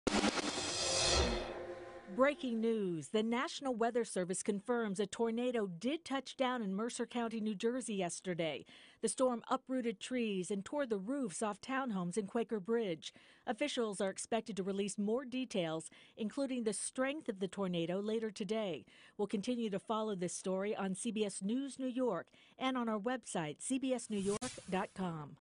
Tornado confirmed in Mercer County, N.J.
The National Weather Service confirmed a tornado tore through the Quaker Bridge area during Tuesday's storm.